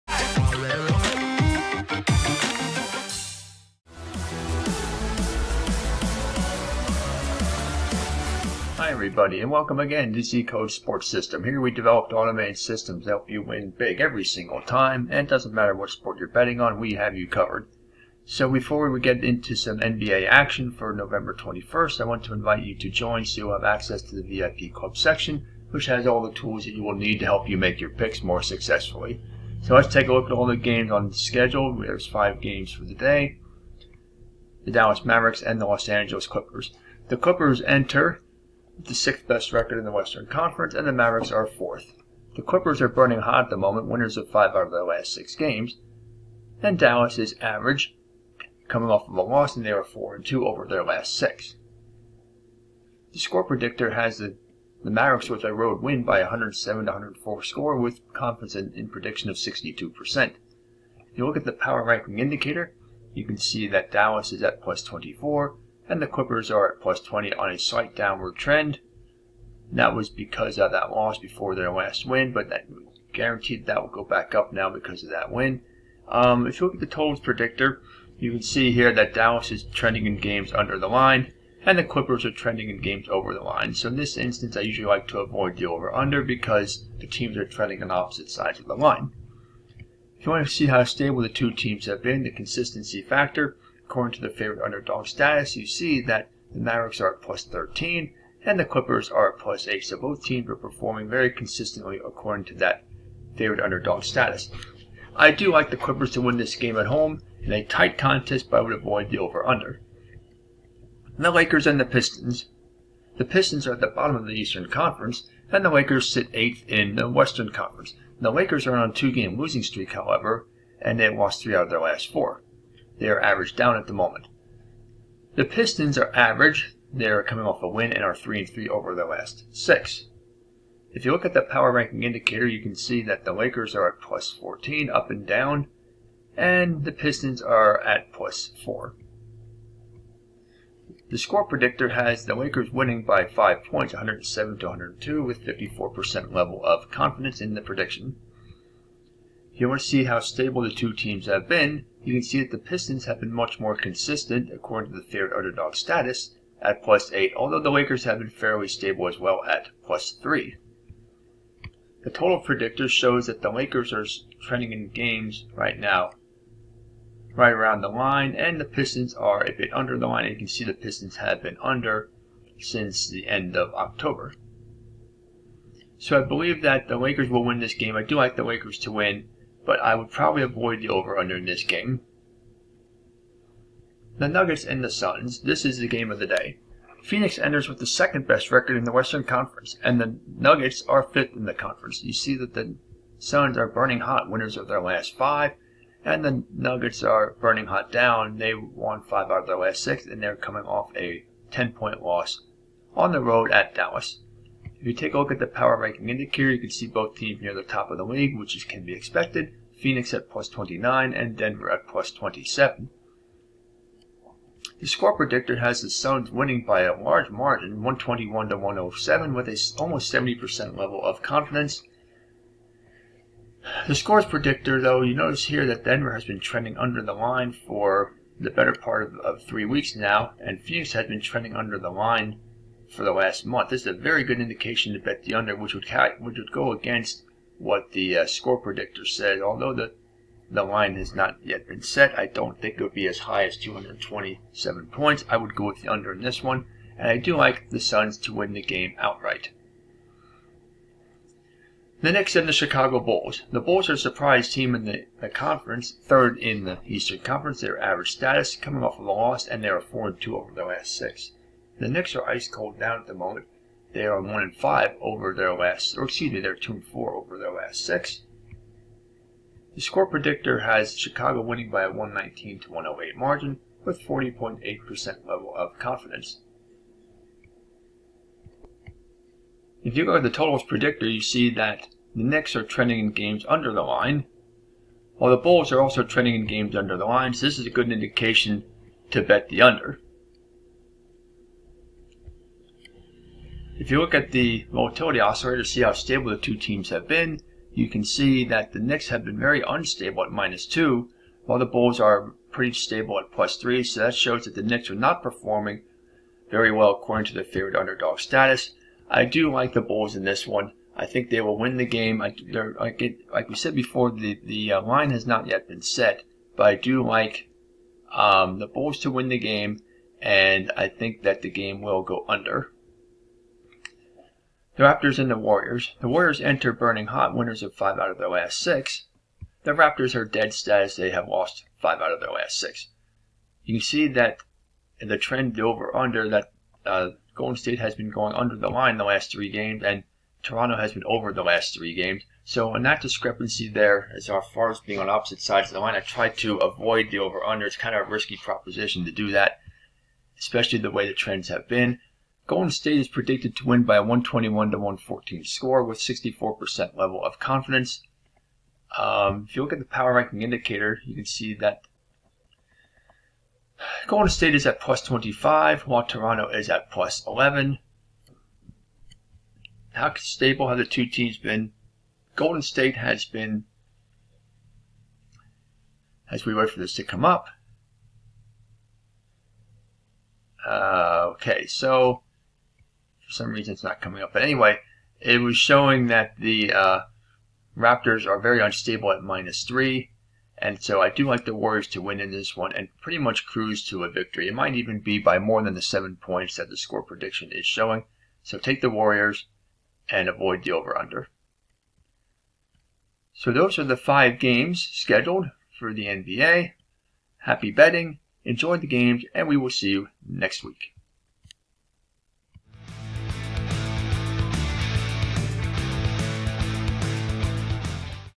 NBA Picks November 21: Nuggets and Suns Battle in Featured Game
With 20 percent of the NBA schedule in the rearview mirror, there are plenty of battles shaping up for division and conference supremacy. There are five games scheduled for November 21 with the Nuggets and Suns leading the way.

00:00 Lets Start!
00:35 Dallas Mavericks versus Los Angeles Clippers
01:54 Los Angeles Lakers versus Detroit Pistons
03:07 Denver Nuggets versus Phoenix Suns
04:18 New York Knicks versus Chicago Bulls
05:33 Toronto Raptors versus Golden State Warriors